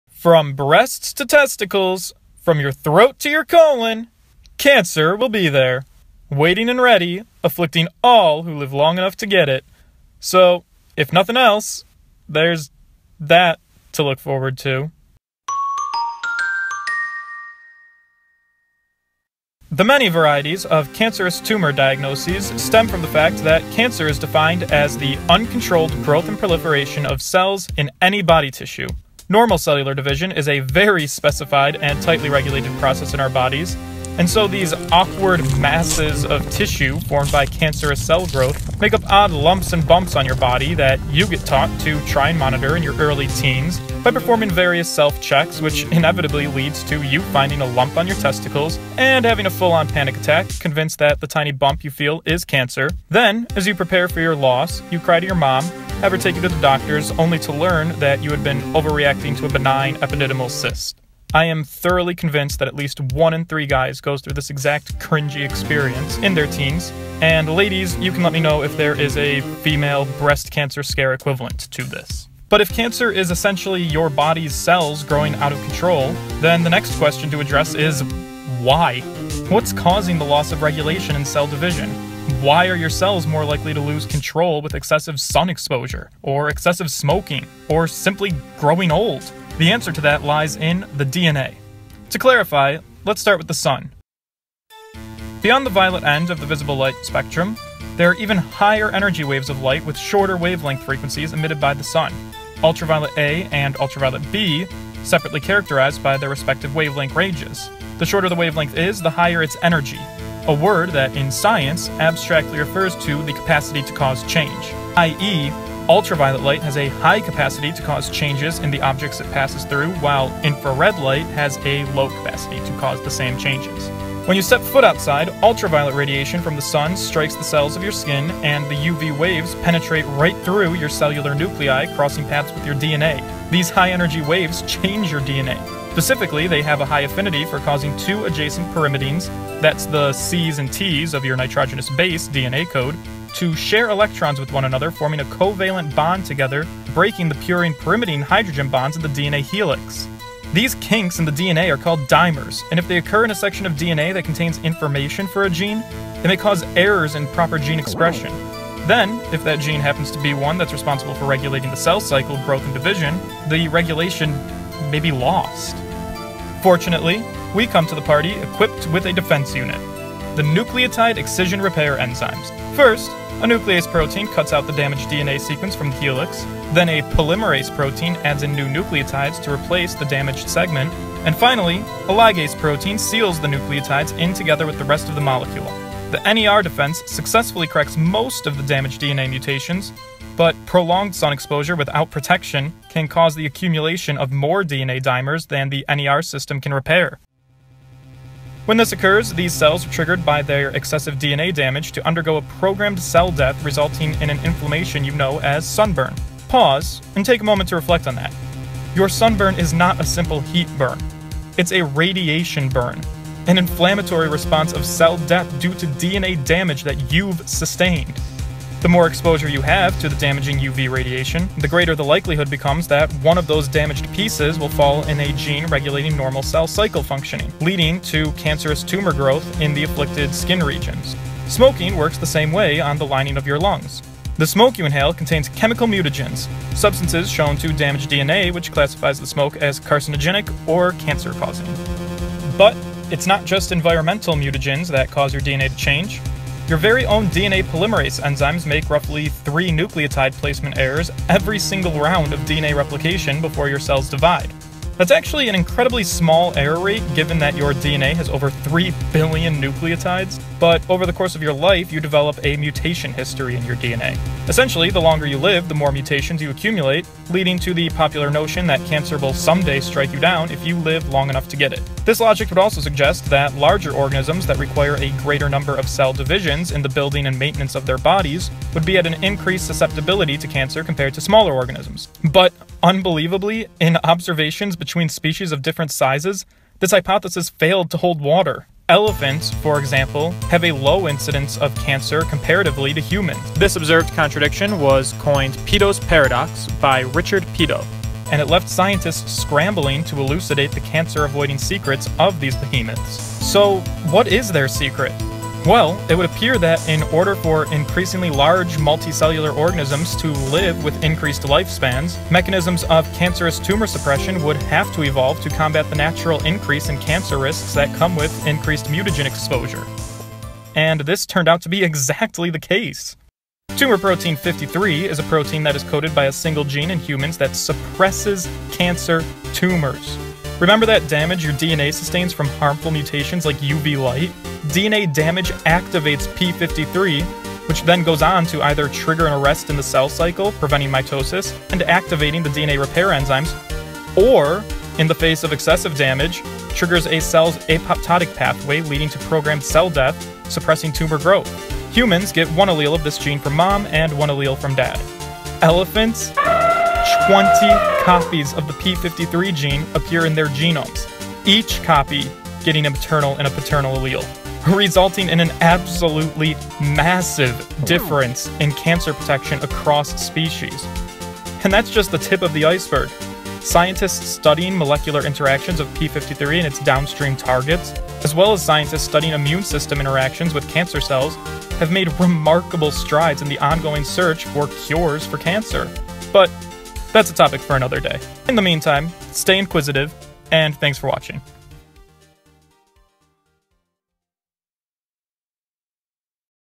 What is Cancer?
The summer months are upon us. As many gear up for long days sunbathing at the beach knowing the risk of melanoma from UV exposure what is REALLY happening at the molecular level?